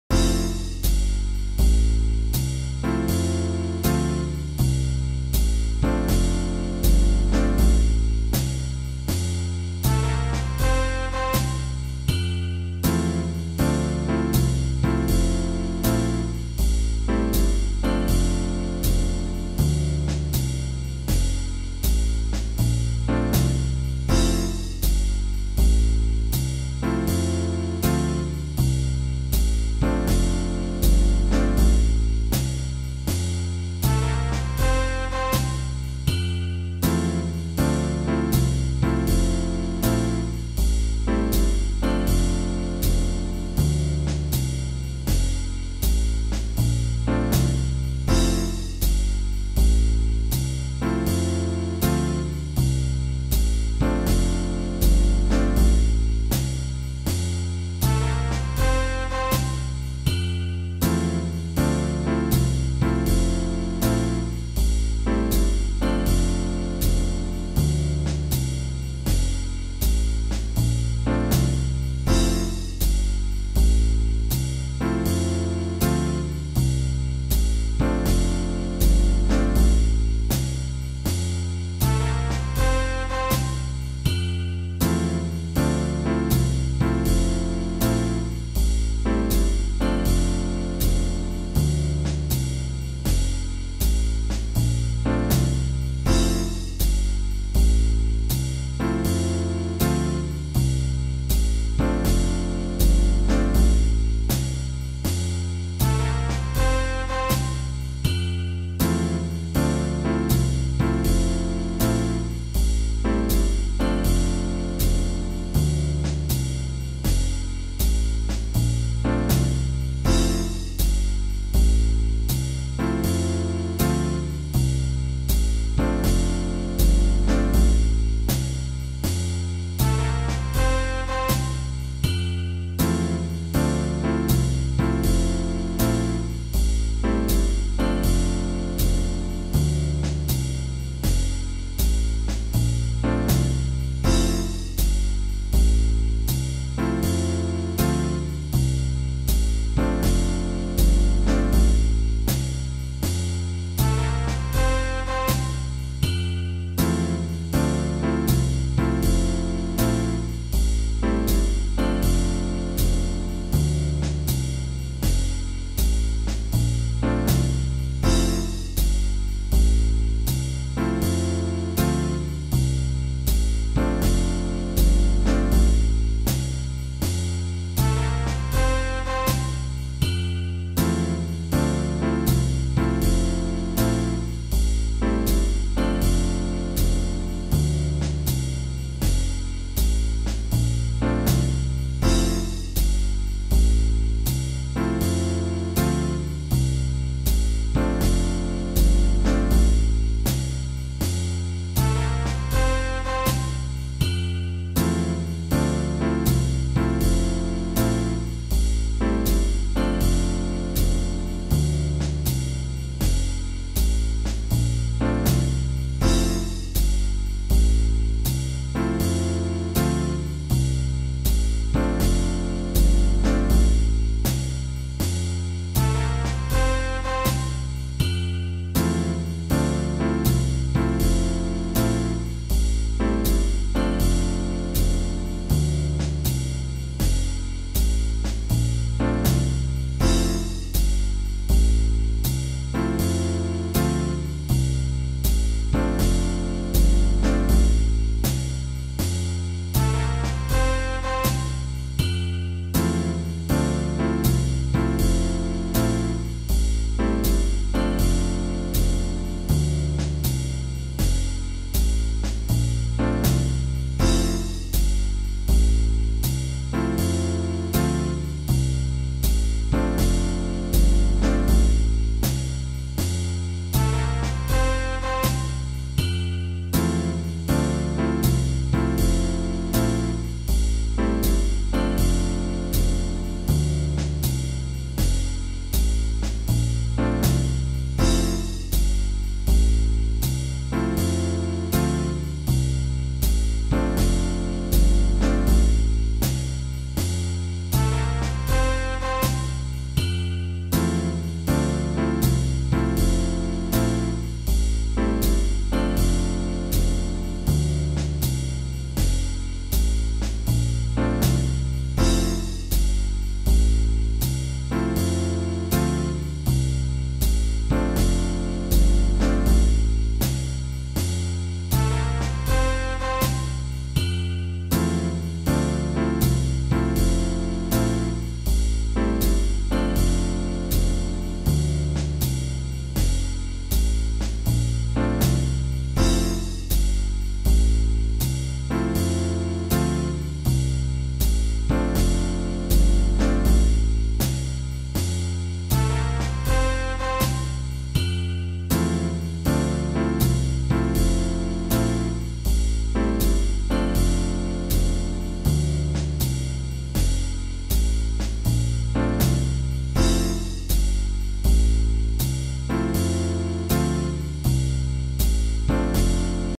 F Major - One Chord Vamp - Jazz M.M.=80
One chord jam to practice with or do with as you wish. Woohootiehoo~! TALKING WITH THE GUITAR